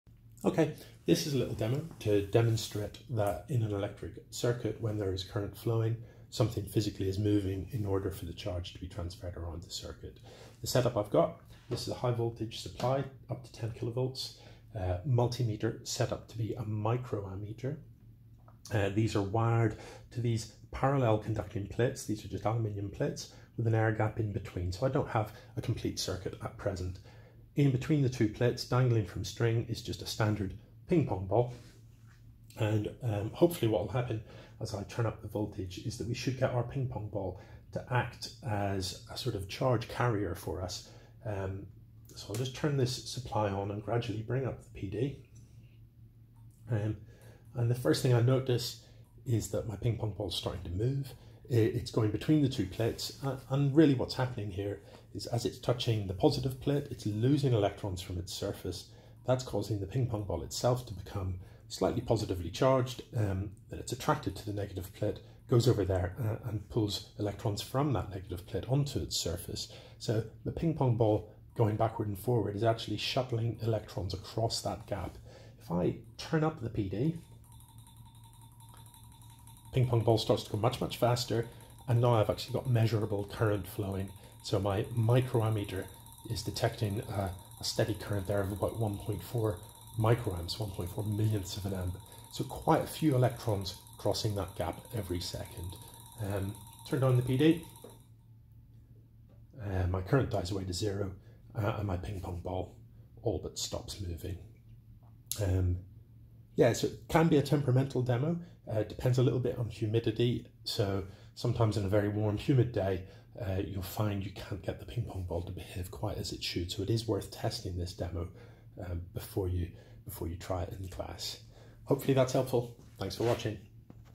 Electric Current Demo 1 - Ping Pong Ball
A straightforward demo to show that when electric current flows, something must physically move in order to transfer the charge. Normally it's electrons flowing through a metal but in this case, it's a ping pong ball because they're much easier to see than electrons!